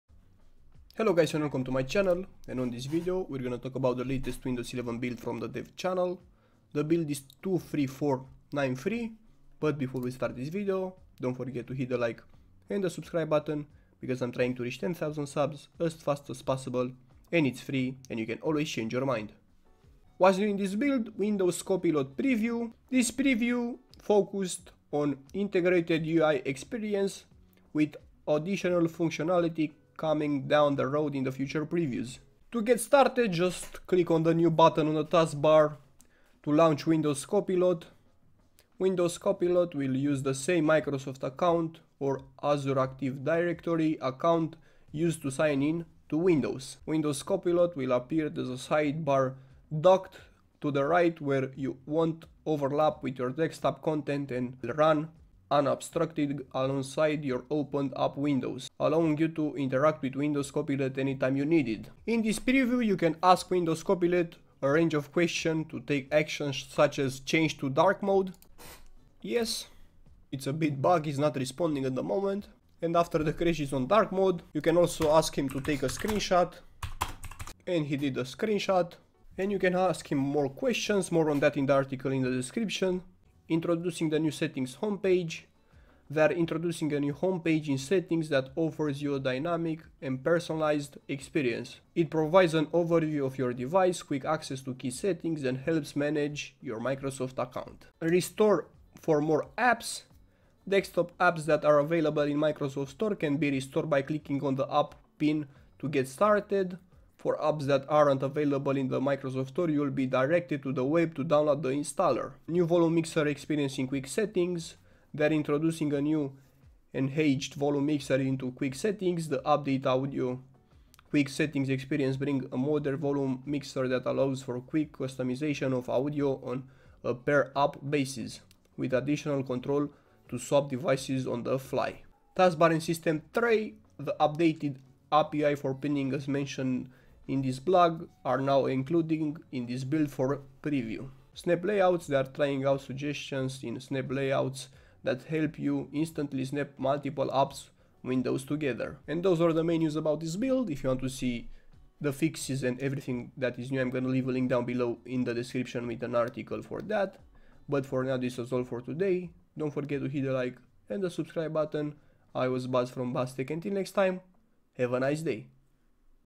New Windows 11 Build 23493 - New Settings Homepage, New Windows Copilot AI
If this video was helpful, don't forget to leave a LIKE and SUBSCRIBE to the channel with the notification bell activated so you won't miss any future uploads from this channel.